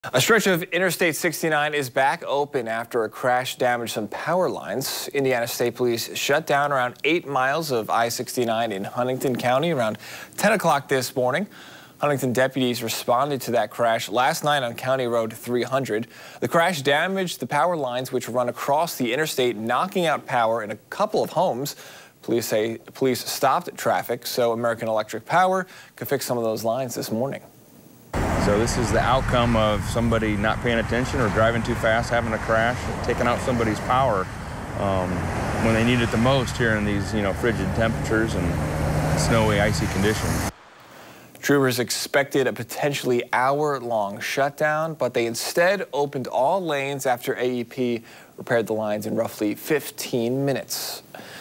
I-69 in Markle reopen after power lines repaired
Indiana State Police shut down roughly eight miles of I-69 in Huntington county around 10:00 a.m. Friday morning.